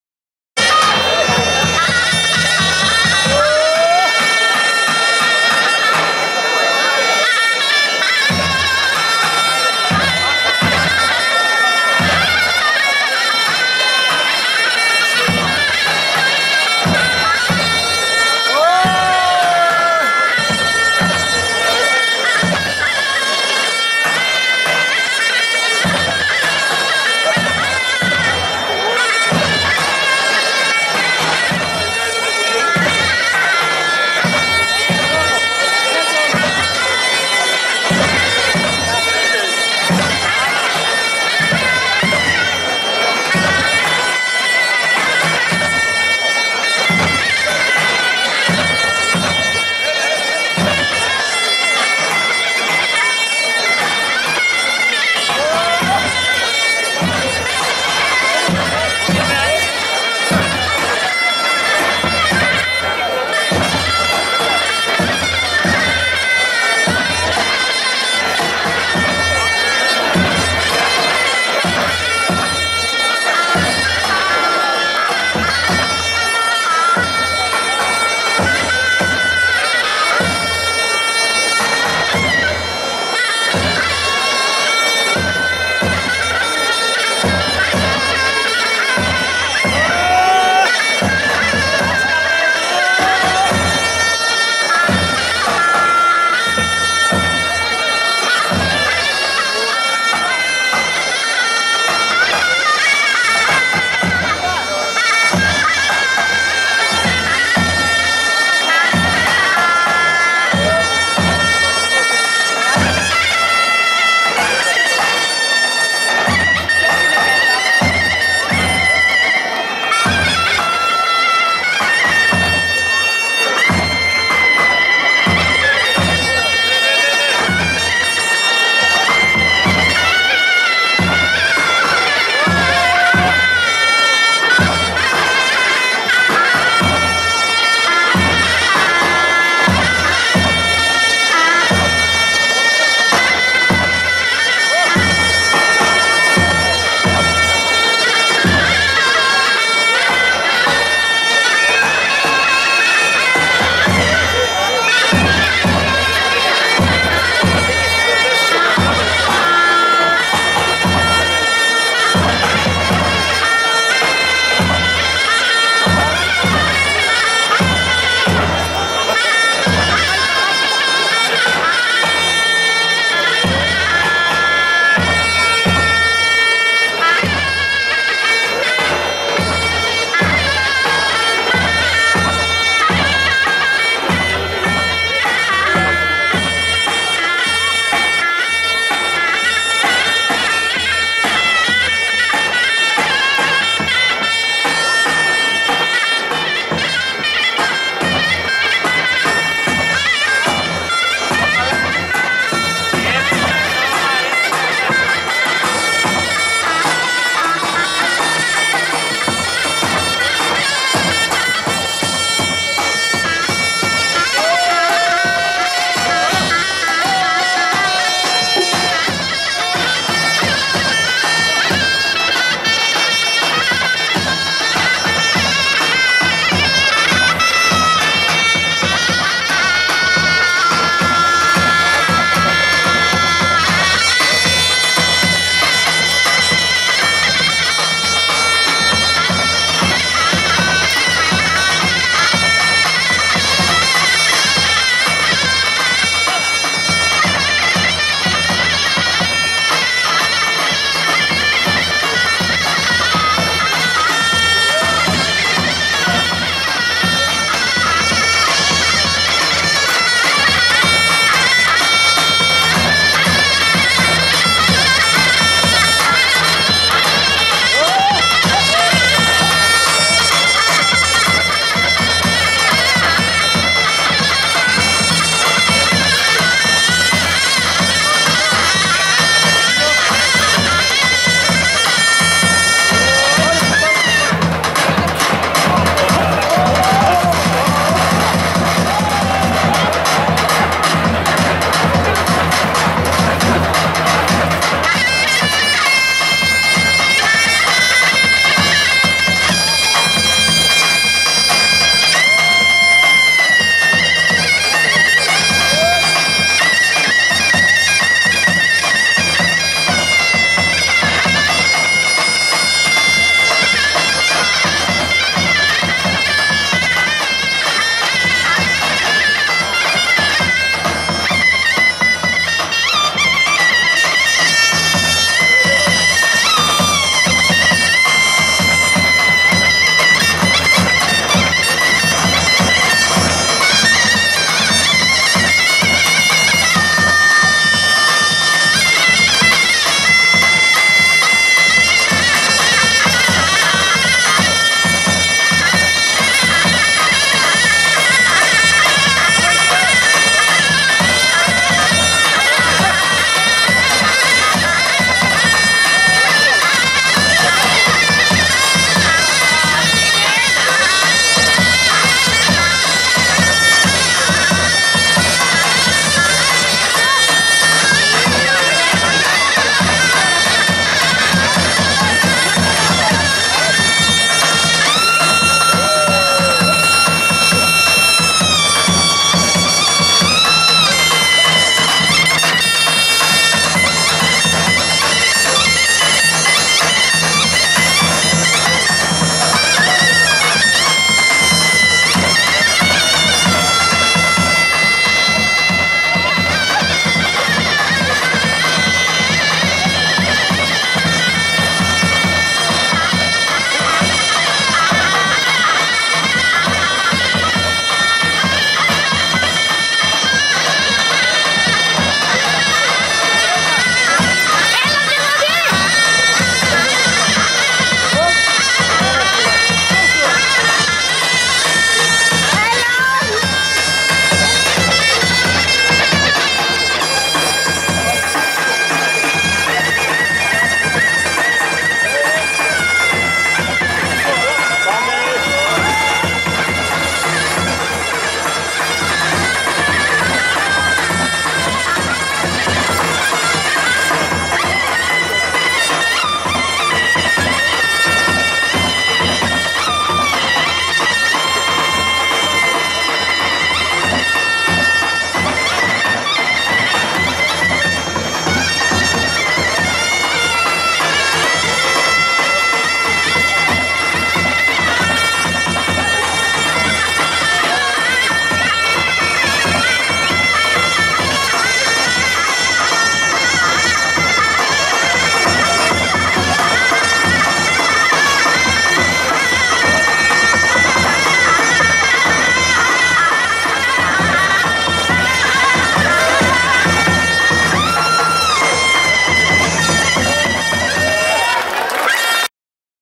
ΓΚΑΪΝΤΑ Βαμβακοφύτου # ζουρνάδες ΔΡΑΜΑΛΗ # Τζαμάλα 2024 ΠΥΡΡΟΥ
*** Στην 11η Τζαμάλα του Εργαστηρίου Λαογραφικών Μελετών και Παραδοσιακών Χορών "Πύρρος" στις Σέρρες διασταύρωση Νικομηδείας και Στρυμώνος (Κυριακή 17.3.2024).